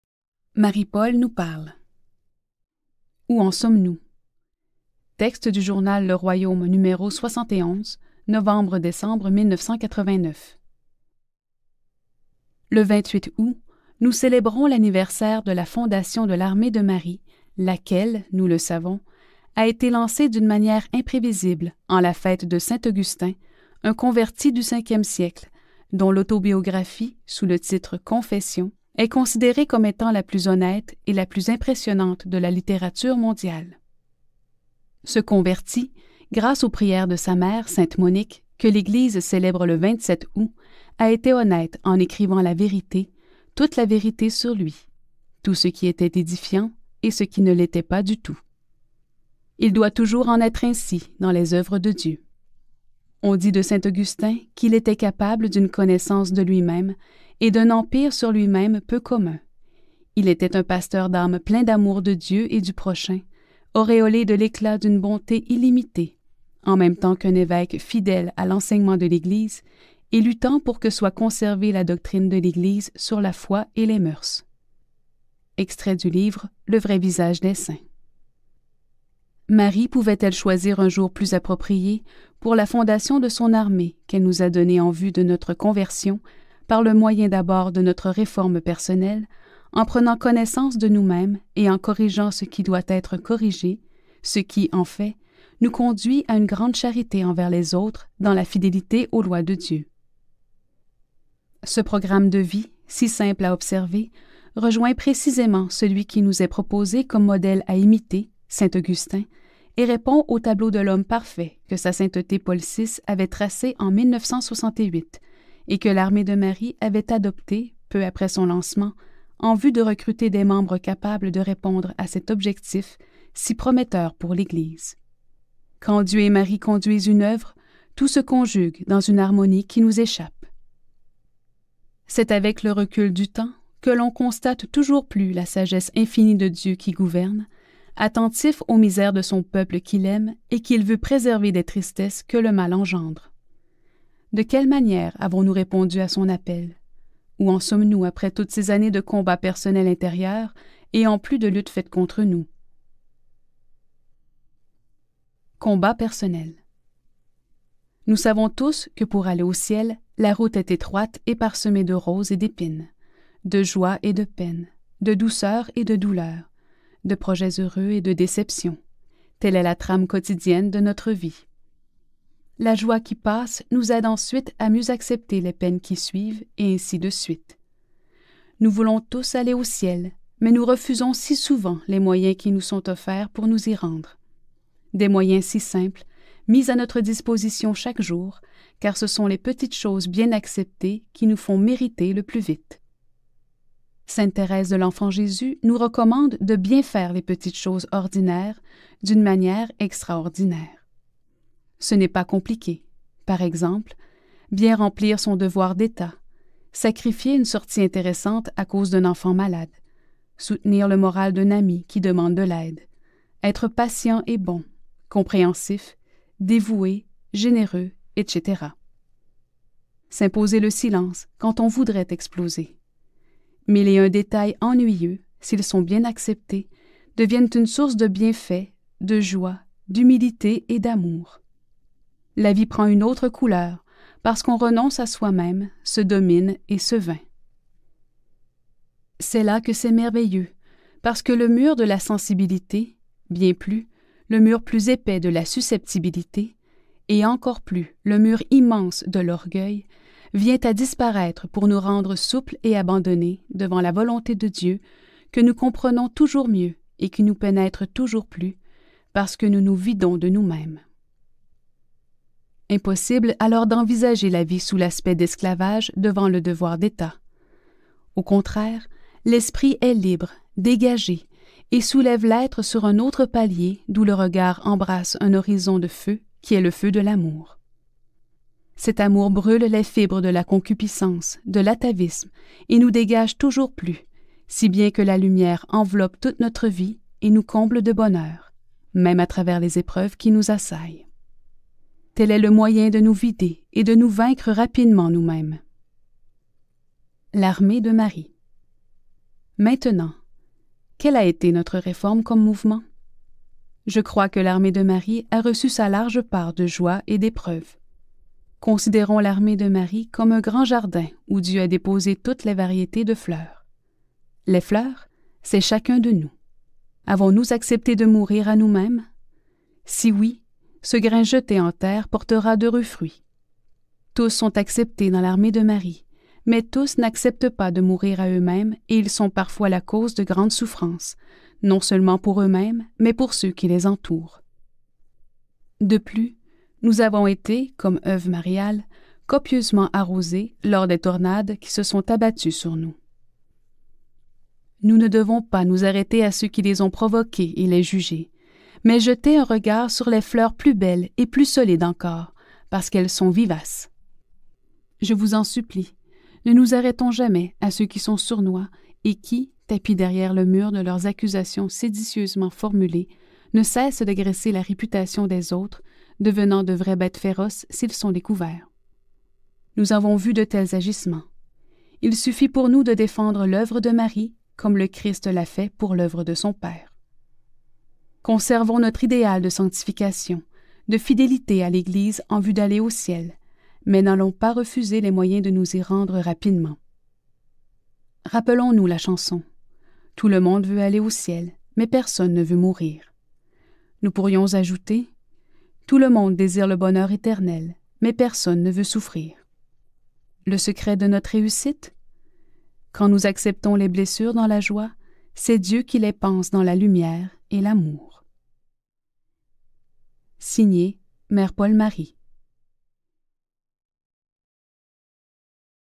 09 - Marie-Paule nous parle - "Où en sommes-nous?" - 1989
Nous présentons des textes écrits par Mère Paul-Marie parus dans le journal Le Royaume dans les années 80 et 90 afin de tirer profit de ses exhortations maternelles et de son précieux enseignement.
09 - "Où en sommes-nous?",  article tiré de la revue Le Royaume, no 71, novembre-décembre 1989